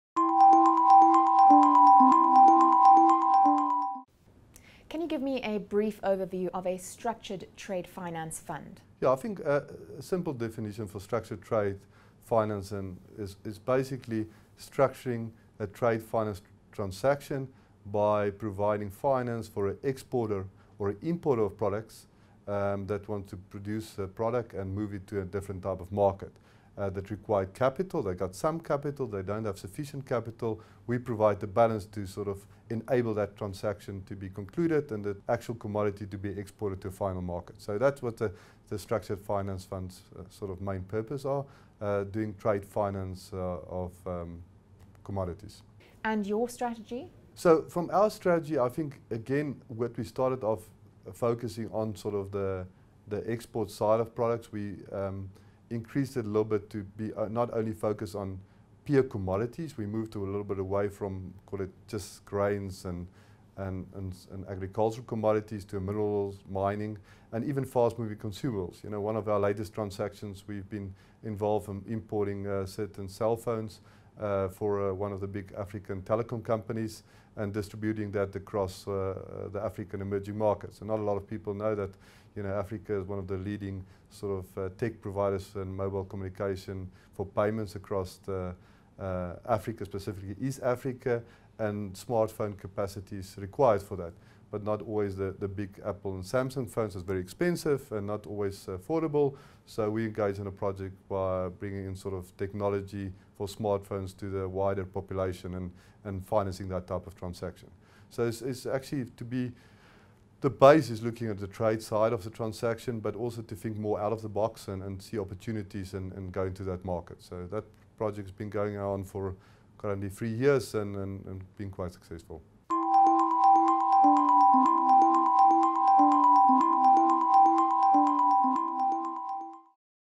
Barak - What is a structured trade finance fund? - BLACK ONYX
BLACK ONYX interviews award winning asset managers. Here we discuss Barak Funds with Co-founder and Chief Investment officer Prieur de Plessis.